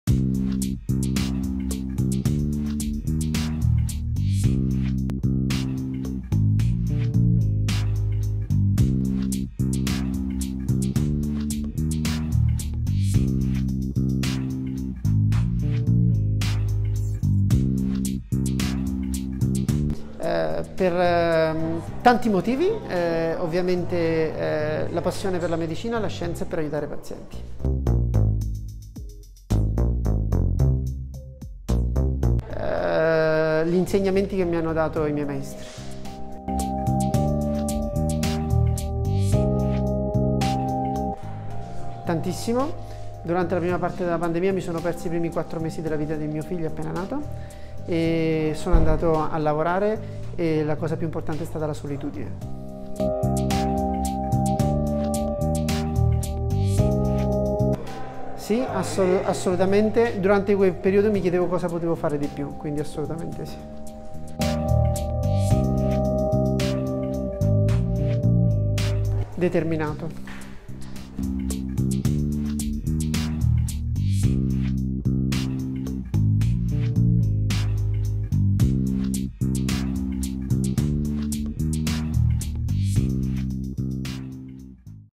ROBERTO IACOVELLI - Oncologia Medica Fondazione Policlinico Universitario “A. Gemelli” (Roma)
Avete mai chiesto ad un medico perché ha scelto questa professione? Noi sì.

Insieme ad altre domande abbiamo provato a conoscere il lato umano di chi c'è dietro una mascherina chirurgica.

Questo progetto nasce con l'intento di evidenziare l'altra parte, quella spesso nascosta, di medici, infermieri e del personale sanitario in generale affinché non ci si ricordi di loro solo nei momenti di pandemia o difficoltà ma sempre.

Rispettare chi ha anche perso la vita durante il periodo Covid-19 e del loro imprescindibile ruolo nella nostra società.

Questa iniziativa è no profit e non ha nessuna sponsorizzazione se non quella del nostro cuore come ringraziamento.